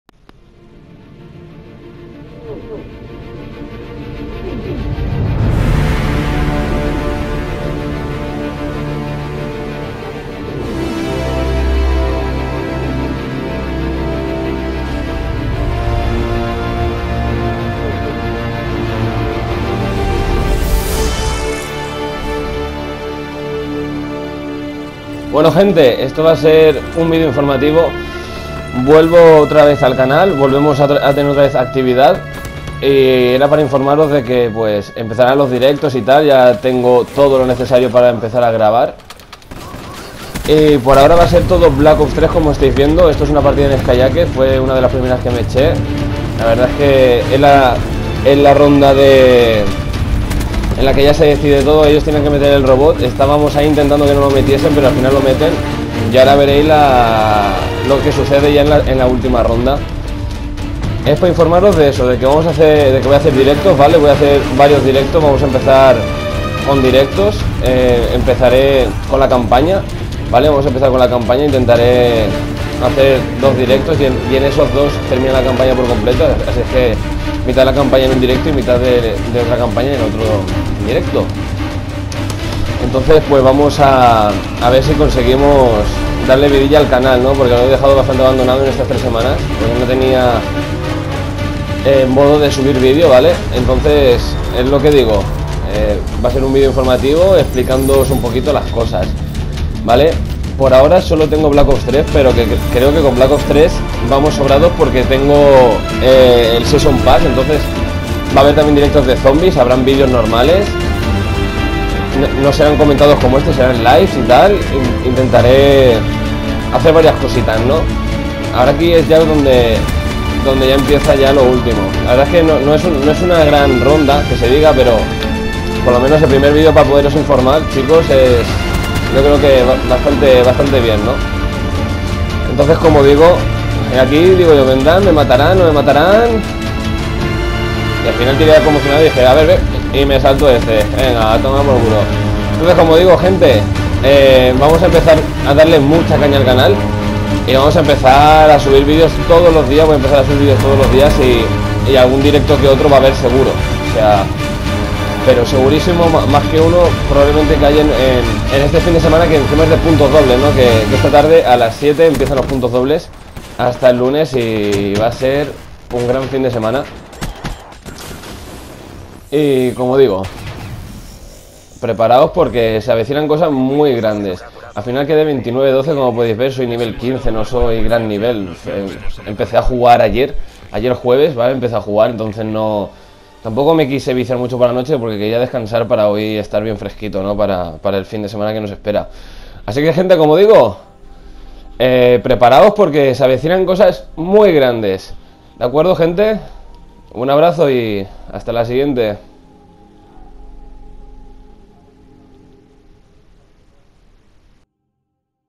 Vuelvo a estar activo,directos e informacion del canal!!
Gente la espera a llegado a su fin!!! Vuelvo mas fuerte que nunca!!! Se avecinan directos de bo3 y mucho mas. LIKE Y SUBSCRIBETE SI NO LO ESTAS!!!